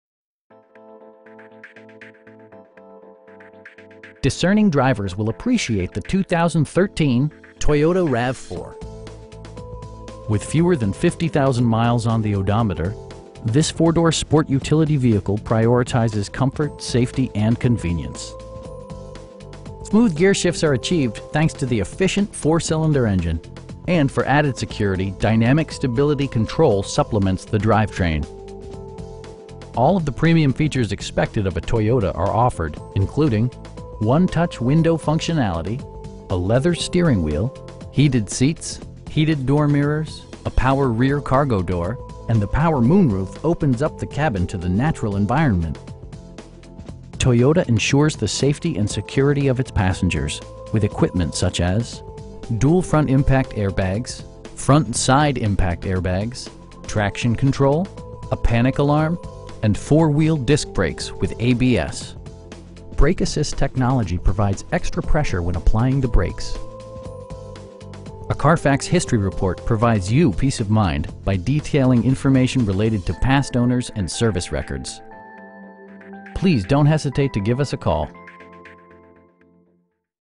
2013 Toyota RAV4 Limited in Fort Worth, TX 76116
3451 West Loop 820 S.

Discerning drivers will appreciate the 2013 Toyota RAV4. With fewer than 50,000 miles on the odometer, this 4 door sport utility vehicle prioritizes comfort, safety and convenience. Smooth gearshifts are achieved thanks to the efficient 4 cylinder engine, and for added security, dynamic Stability Control supplements the drivetrain. All of the premium features expected of a Toyota are offered, including: 1-touch window functionality, a leather steering wheel, heated seats, heated door mirrors, a power rear cargo door, an overhead console, and the power moon roof opens up the cabin to the natural environment. Toyota ensures the safety and security of its passengers with equipment such as: dual front impact airbags, front SIDE impact airbags, traction control, a panic alarm, and 4 wheel disc brakes with ABS. Brake assist technology provides extra pressure when applying the brakes. A Carfax history report provides you peace of mind by detailing information related to past owners and service records. We pride ourselves in consistently exceeding our customer's expectations. Please don't hesitate to give us a call.